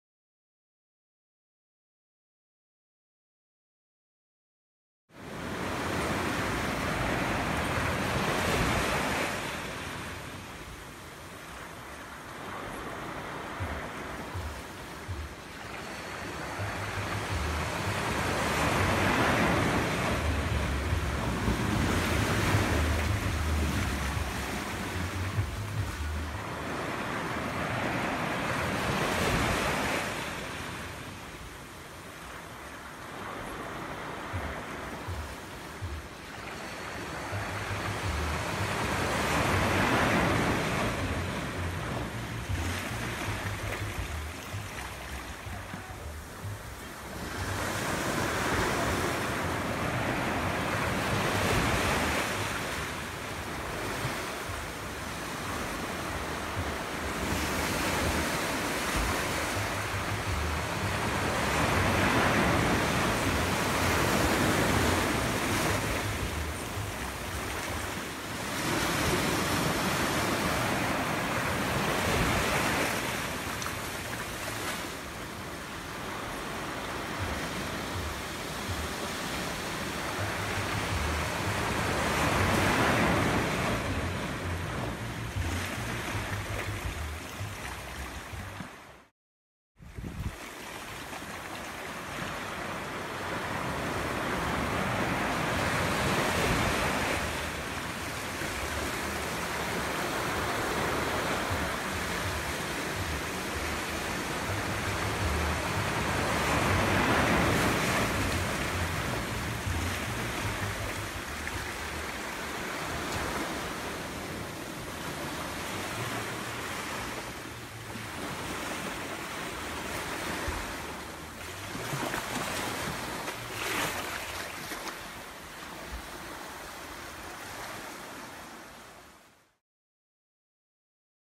Cabbage White Caterpillers & Butterflies
Cabbage White caterpillars and butterflies found on Villa Beach, St. Vincent and the Grenadines.

Filmed & edited by:  L. Darrien Ollivierre